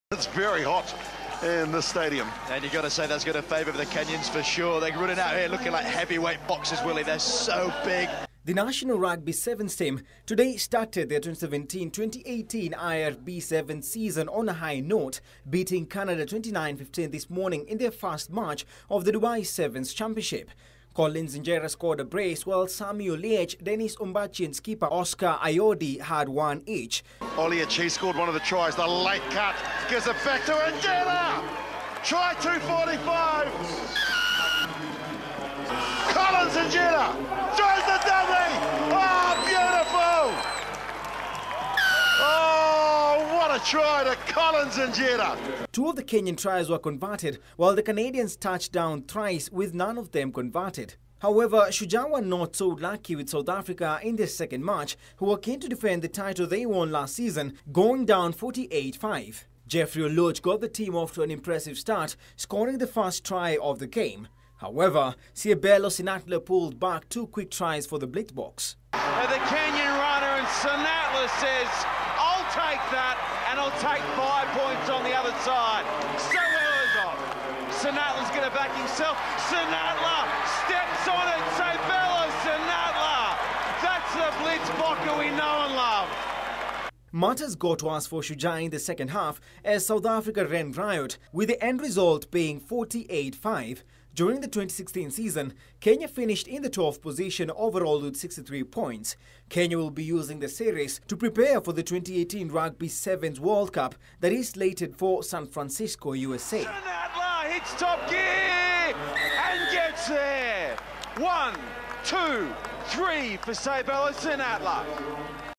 National Rugby Sevens team has qualified for the quarter finals of the Dubai sevens
National Rugby Sevens team has qualified for the quarter finals of the Dubai sevens after beating Uganda 29-14 in their last group match. The team coached by Innocent Simiyu booked a place in the Main Cup quarters after finishing first in group ‘A’ with seven points.